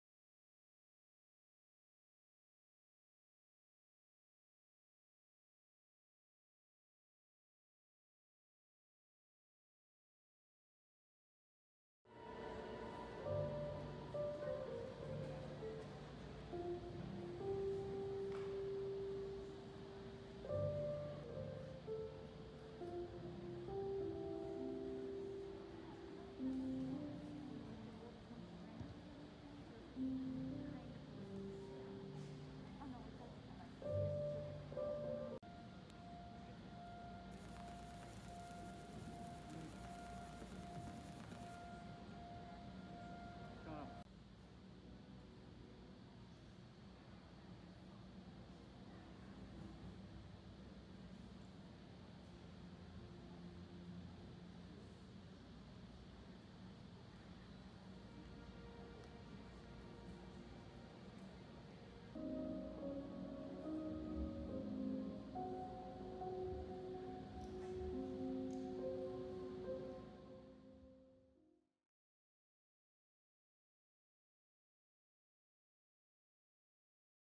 Vincent Van Gogh - Immersive Experience
My time spent at the gigantic interactive venue in NYC